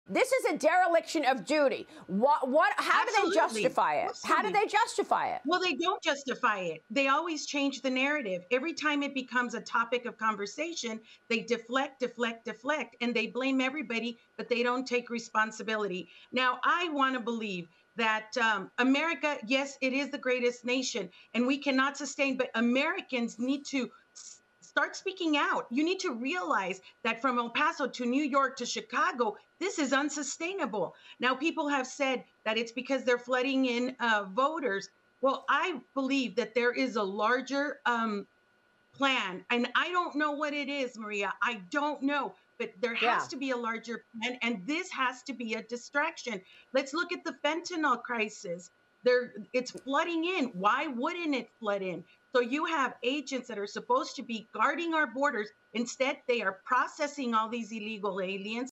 Border expert calls surging immigration crisis a ‘distraction’
Border Security Coalition President Irene Armendariz-Jackson discusses the Biden administration’s handling of the surge in illegal migrants.